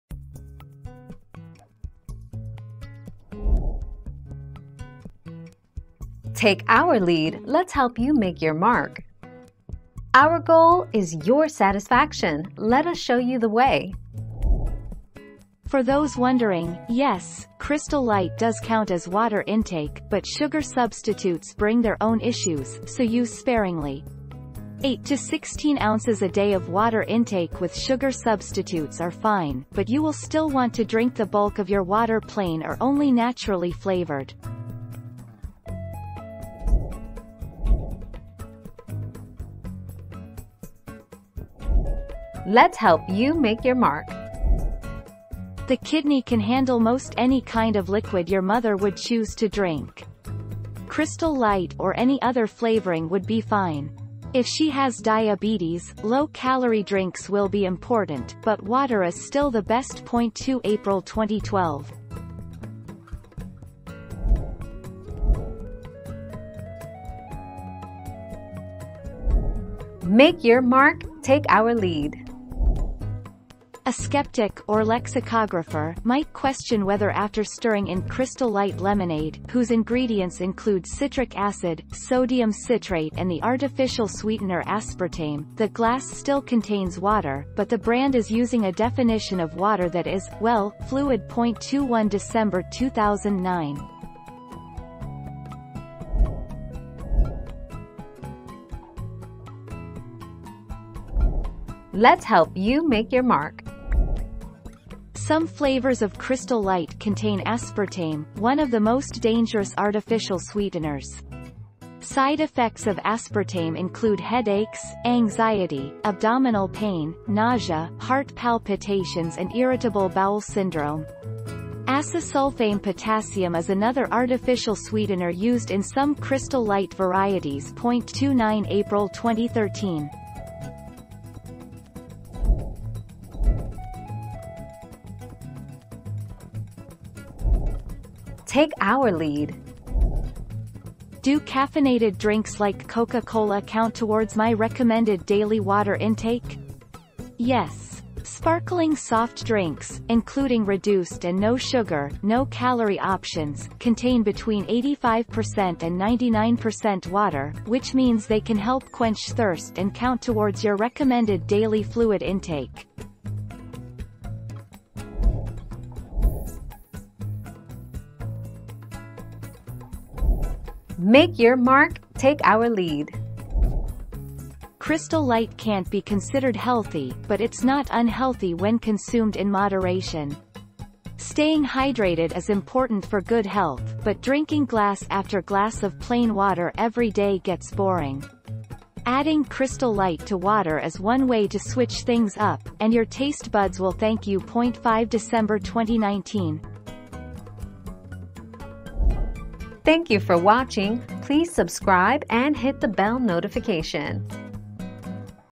Does it still count as water if I put crystal light in it?
00:00 - Does it still count as water if I put crystal light in it?
00:38 - Is Crystal Light good for your kidneys?
01:06 - Does Crystal Light lemonade count as water?
01:37 - What are the side effects of crystal light?
02:13 - Does Coke count as water intake?
02:46 - Does Crystal Light make water less healthy?

Laura S. Harris (2021, March 14.) Does it still count as water if I put crystal light in it?
    AskAbout.video/articles/Does-it-still-count-as-water-if-I-put-crystal-light-in-it-228463